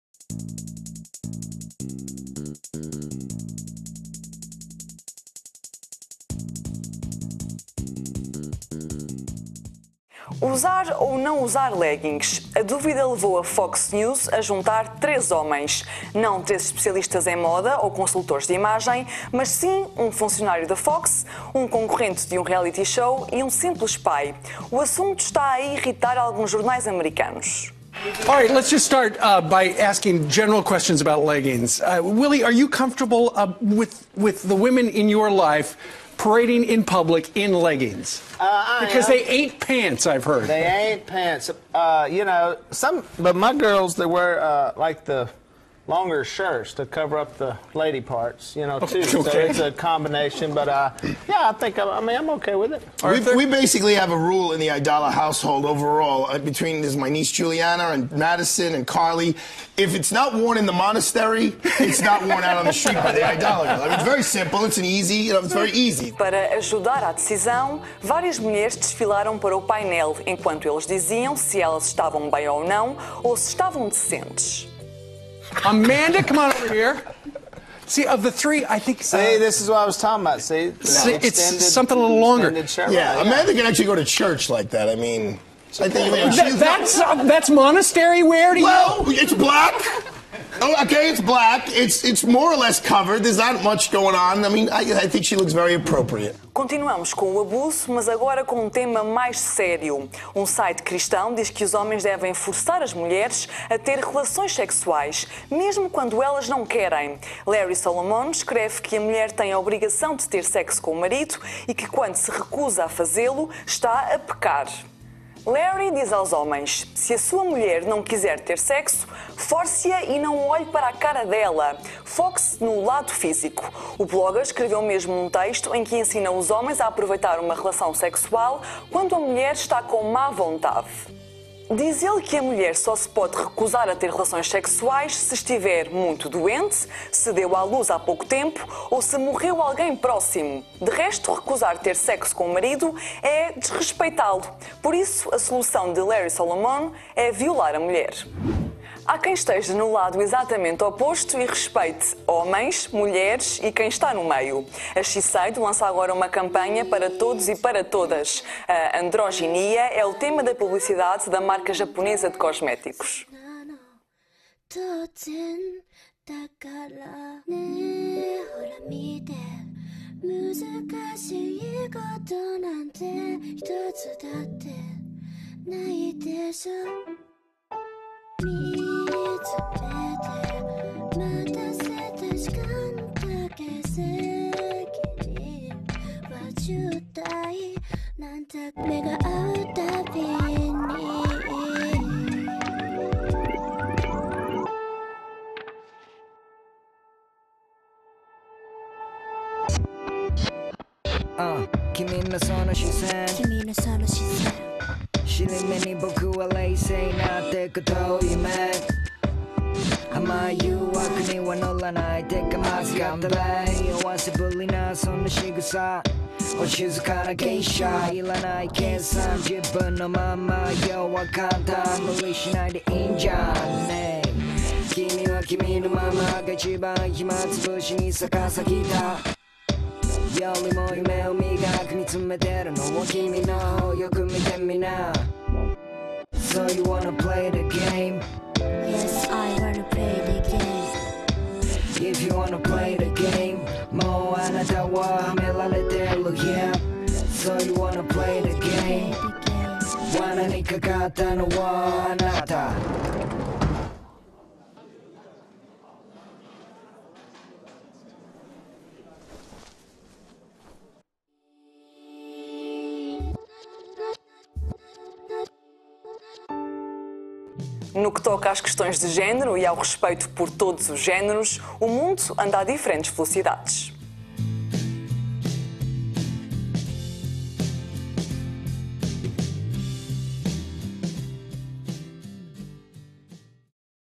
Leggings, o abuso sexual e a surpresa
A Fox fez um debate sobre leggings com mulheres a desfilar para homens. Um site cristão diz que os maridos devem forçar as mulheres a ter sexo e ensina como. Mas as imagens enganam. Confusos?